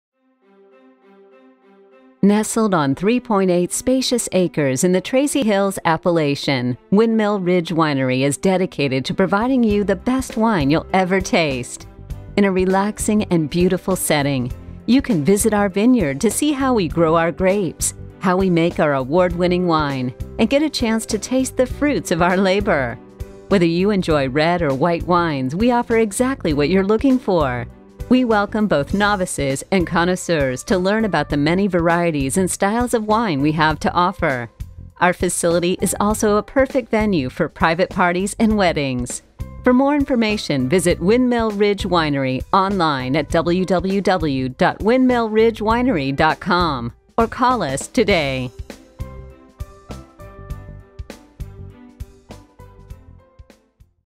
Windmill Ridge Winery | Wineries & Vineyards in Tracy
Nestled on 3.8 spacious acres in the Tracy Hills Appellation, Windmill Ridge Winery is dedicated to providing you the best wine you'll ever taste! In a relaxing and beautiful setting, you can visit our vineyard to see how we grow our grapes, how we make our award-winning wine, and get a chance to taste the fruits of our labor.  Whether you enjoy red or white wines, we offer exactly what you’re looking for. We welcome both novices and connoisseurs to learn about the many varieties and styles of wine we have to offer. Our facility is also a perfect venue for private parties and weddings!